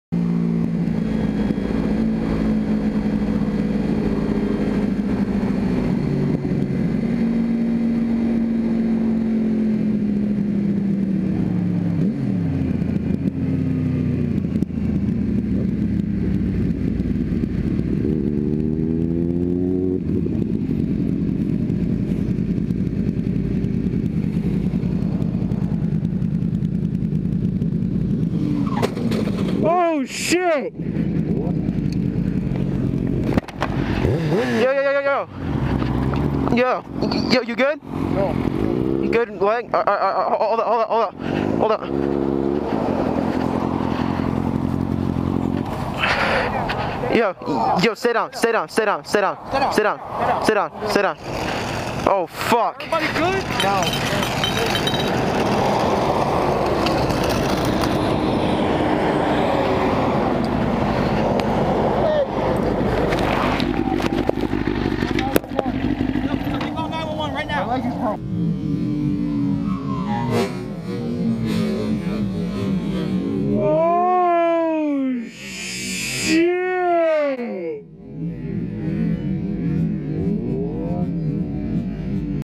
MOTORCYCLE CRASH 3 BIKE PILE UP
rider 1 went to hospital with broken leg,
rider 2 status unknown
rider 3 walked away